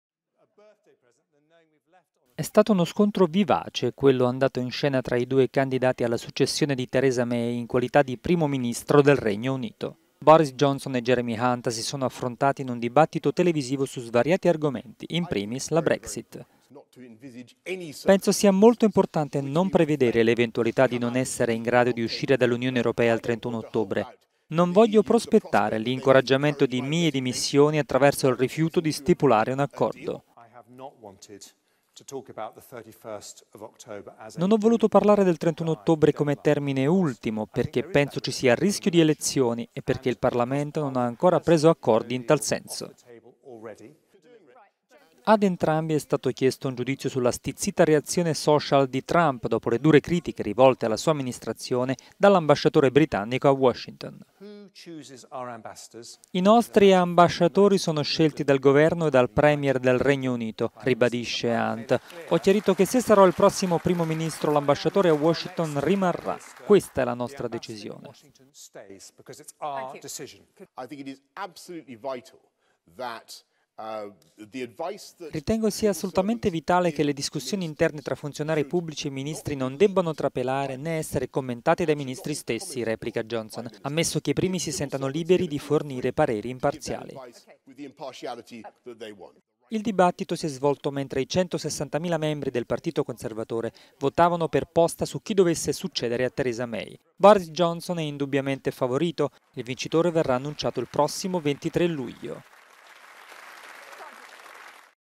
Regno Unito: dibattito tv tra Hunt e Johnson per succedere a Theresa May
Vivace incontro-scontro tra i candidati a diventare il prossimo leader politico del Regno Unito …